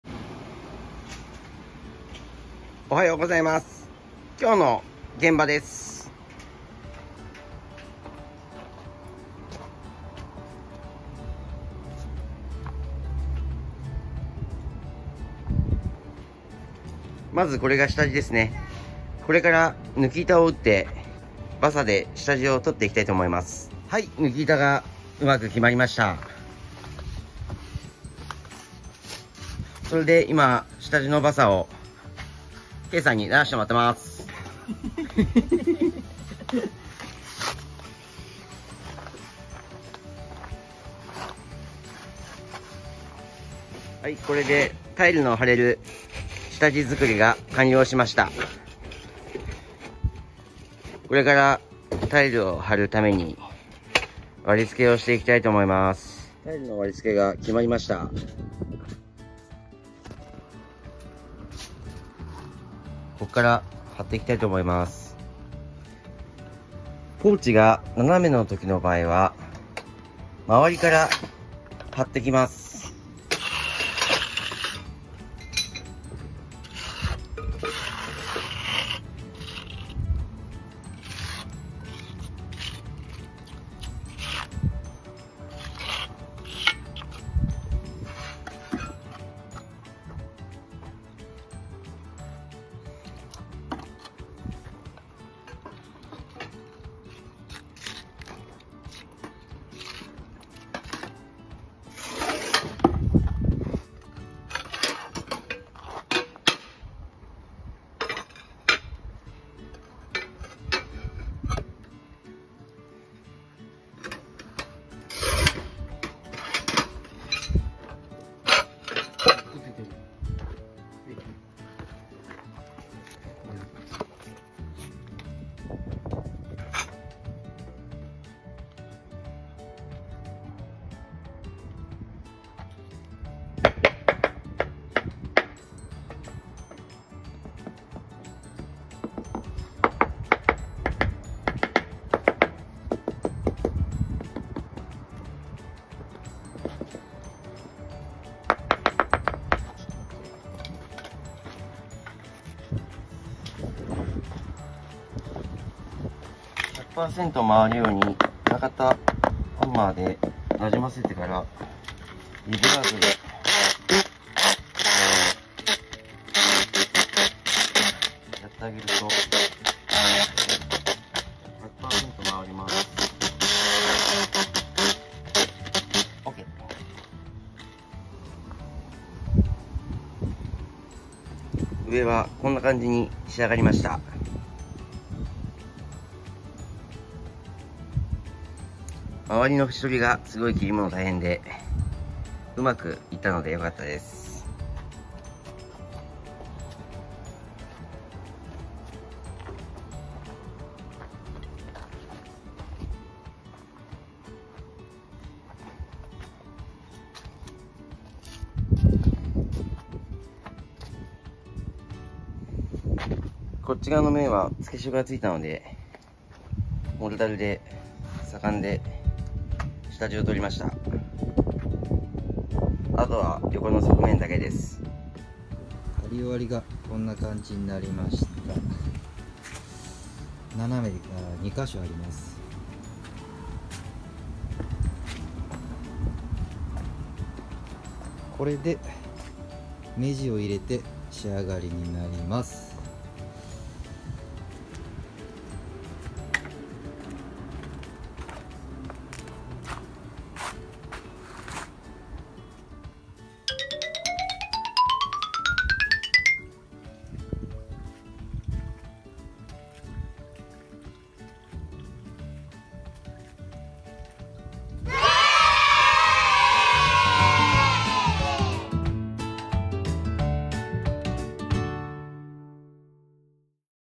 12:【斜めでも、収めます】
今回はさらっと流しの動画でしたが、
これから、
割り付けや切り物の収め方などの特集動画を作成していくので是非楽しみにしていてください✨
左官やタイルのよさを広めたい😎❣️

こちらも遊びに来てください😊♪